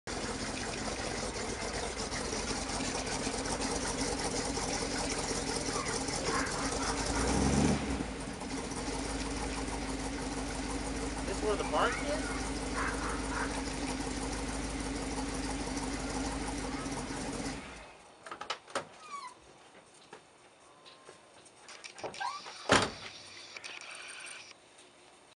rolling smoke 420 dodge dart
showing up to a party in a cloud of 420 smoke rolling out of dodge dart